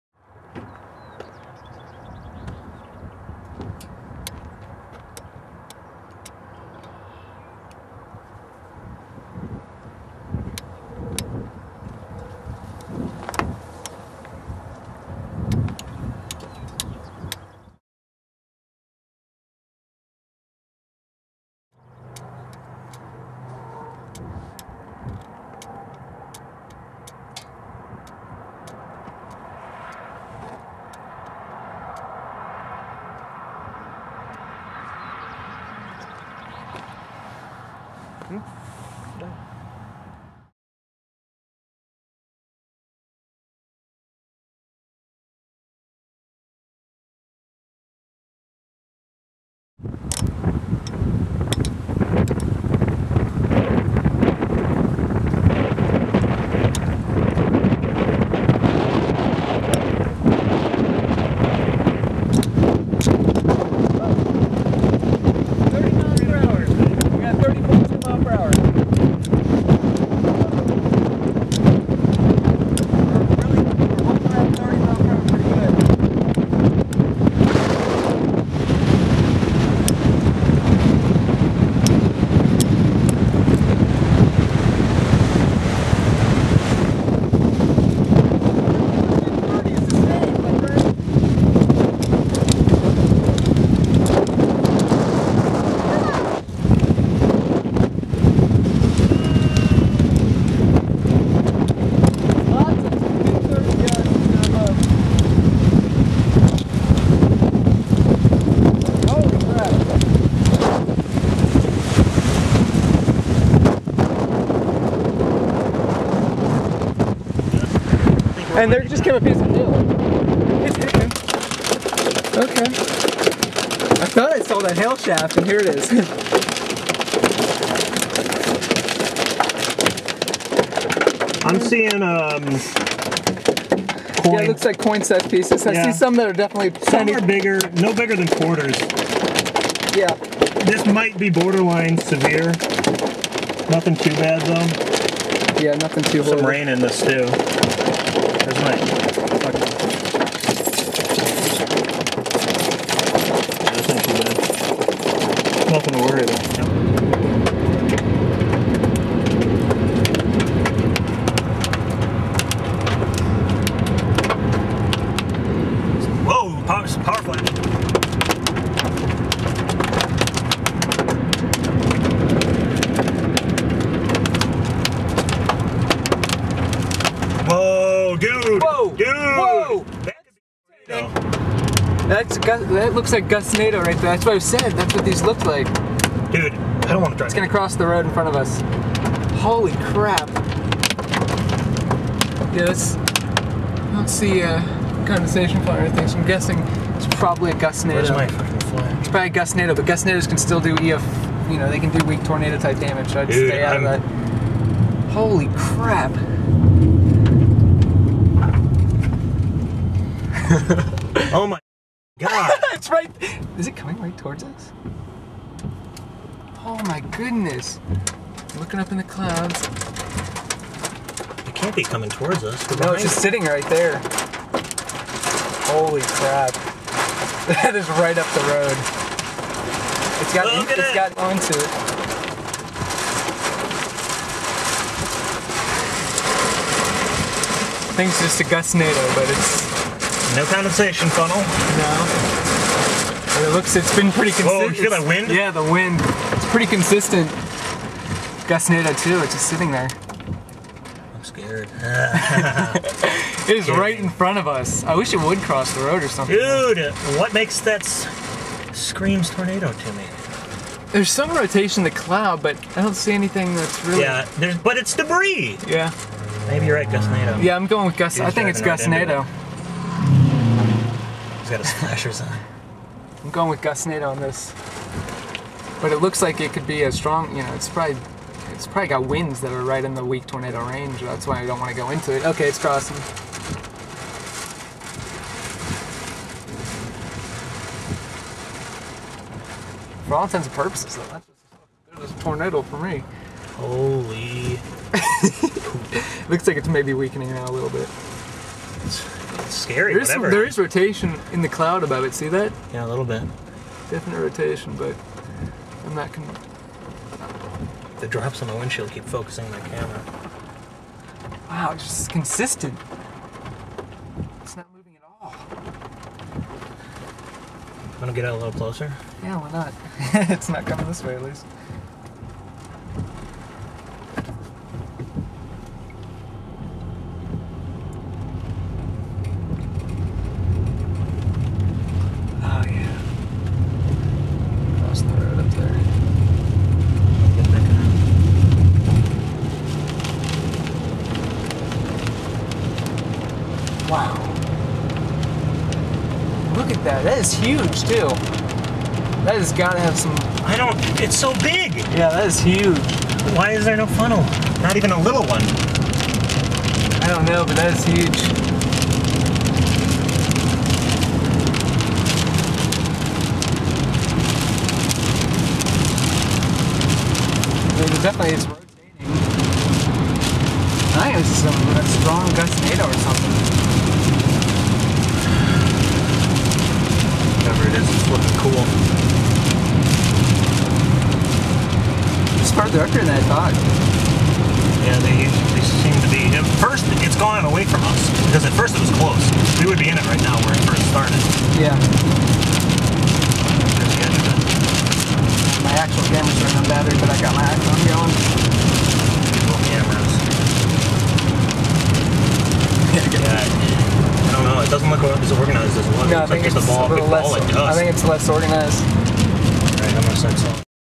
Kansas Gustnado (April 3rd, 2011)
Video of a strong gustnado that formed just outside of Lawrence, KS during the early evening of April 3rd, 2011. According to post-analysis, the gustnado produced wind speeds up to 80mph (equivalent to an EF0 tornado), and did some minor damage.